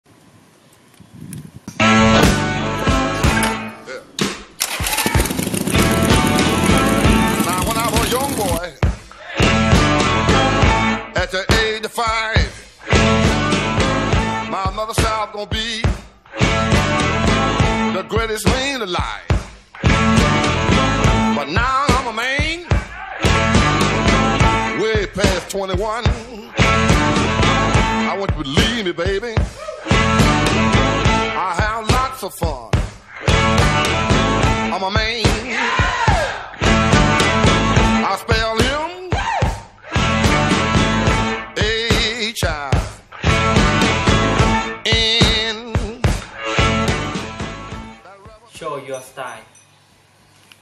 Seiko : Show Your Style - PANLOP
Seiko Present Show Your Style Video Lookbook Contest 2019

By : พัลลภ แก้วใจรักษ์

ร่วมโหวตให้ผู้เข้าแข่งขันกิจกรรม Show Your Style Video Lookbook Contest 2019 โดยผู้เข้าแข่งขันที่ได้ยอดไลค์สูงสุด 5 อันดับแรก จะได้ร่วมเดินแบบในงาน ShowYourStyle พร้อมฟังคำตัดสินจากคณะกรรมการ ในวันที่ 11 ธันวาคม 2019 ในงาน ”Show Your Style” กับ Seiko 5 Sports ณ. ศูนย์การค้าเซ็นทรัลเวิลด์

เปิดโหวตตั้งแต่วันนี้ - 6 ธันวาคม 2019 เวลา 18.00 น.

*** ประกาศผู้เข้ารอบวันที่  7  ธันวาคม 2019 ***
ที่ Facebook : Seiko Club
    Line Official : @Seiko_thailand